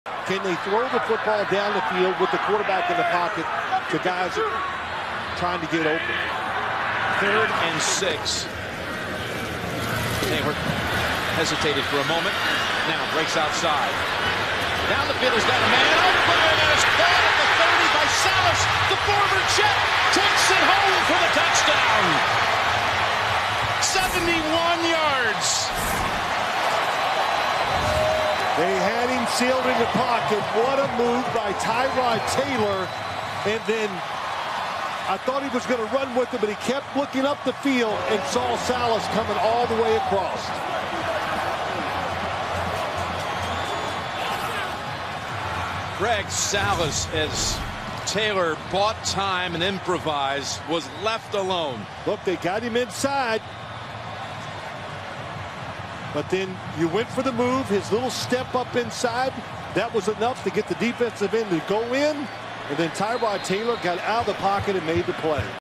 Tyrod Taylor Finds Former Jets WR Greg Salas for 71-Yard TD! | Jets vs. Bills | NFL
Tyrod Taylor uses his speed to escape the pocket and connects with Greg Salas for the 71-yard touchdown! The New York Jets take on the Buffalo Bills in Week 2 of the 2016 NFL season.